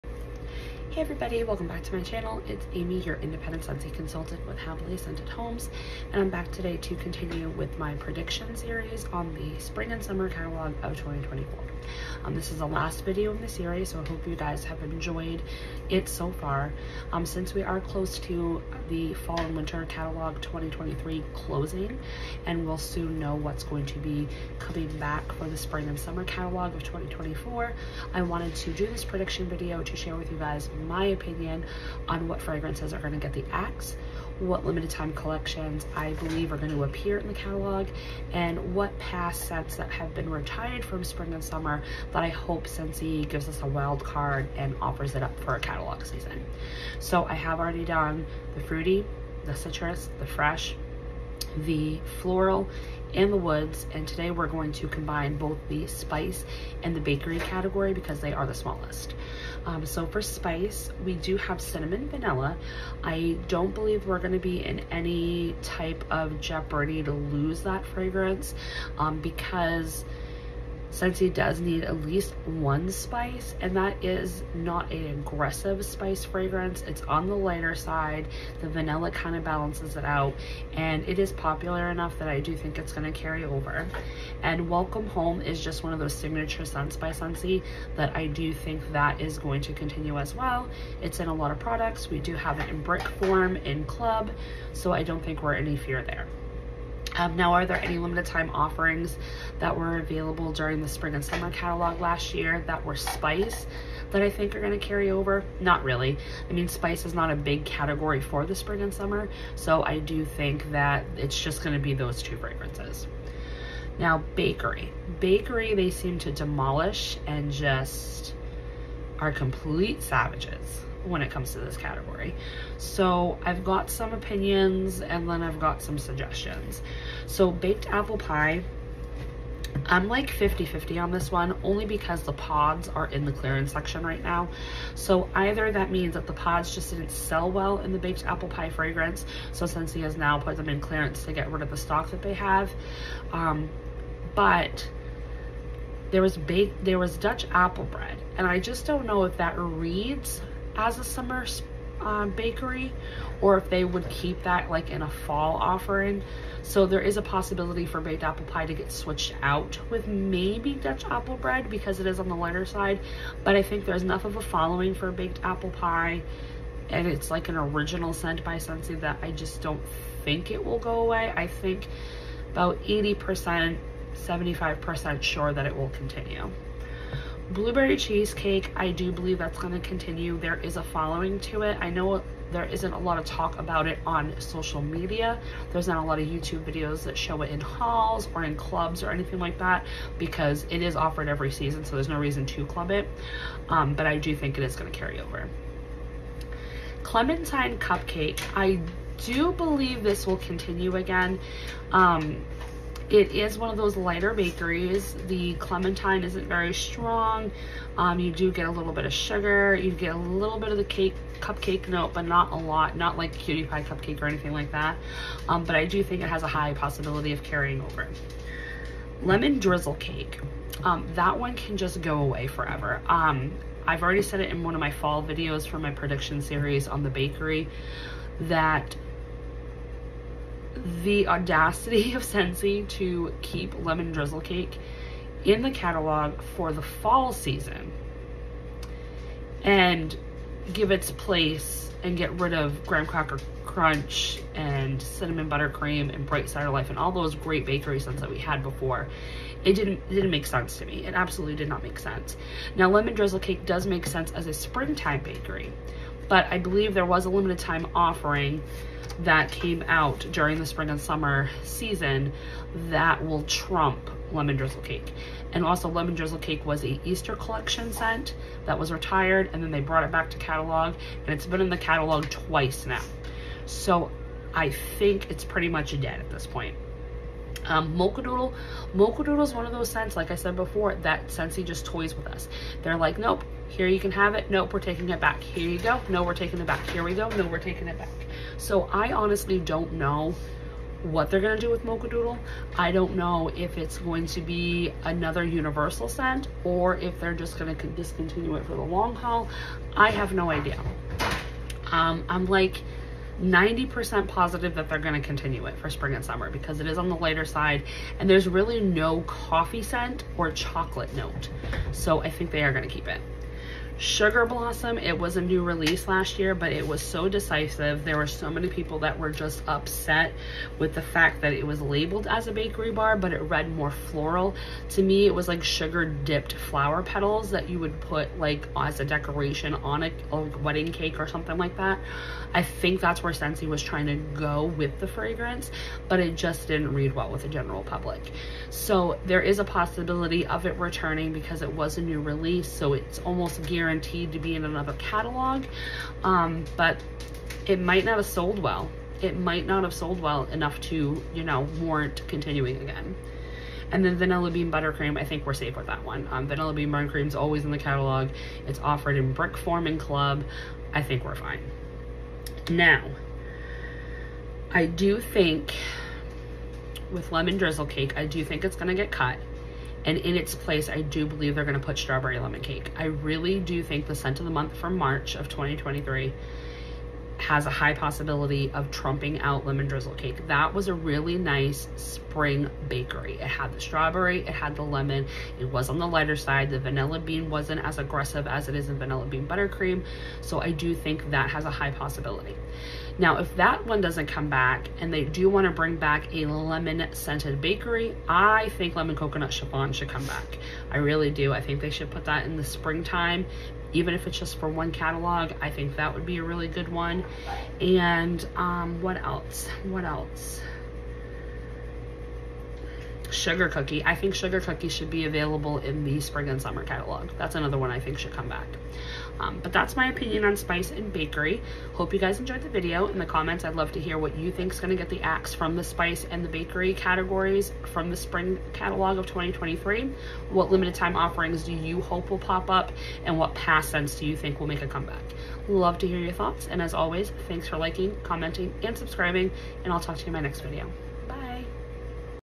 Spring/Summer 2024: Catalog Predictions ( Spice & Bakery)
My name is Aimee and I am an independent Scentsy consultant. In this video, I am discussing my predictions on what catalog fragrances may be coming back in March 2024 for our Spring/Summer catalog